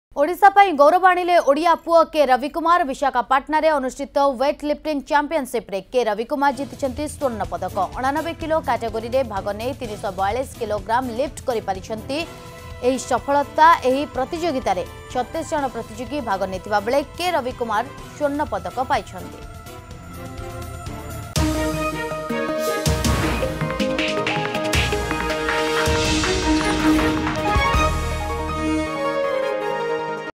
ବିଶାଖାପାଟଣାରେ ଅନୁଷ୍ଠିତ ୱେଟଲିଫ୍ଟିଙ୍ଗ୍ ଚମ୍ପିଆନସିପରେ କେ.ରବିକୁମାର ଜିତିଛନ୍ତି ସ୍ବର୍ଣ୍ଣ ପଦକ
୮୯ କିଲୋ କେଟୋଗୋରୀରେ ଭାଗ ନେଇ ୩୪୨ କିଲୋଗ୍ରାମ୍ ଲିଫ୍ଟ୍ କରି ପାଇଛନ୍ତି ଏହି ସଫଳତା । ଏହି ପ୍ରତିଯୋଗୀତାରେ ୩୬ ଜଣ ପ୍ରତିଯୋଗୀ ଭାଗ ନେଇଥିବା ବେଳେ କେ. ରବିକୁମାର ସ୍ବର୍ଣ୍ଣ ପଦକ ପାଇଛନ୍ତି ।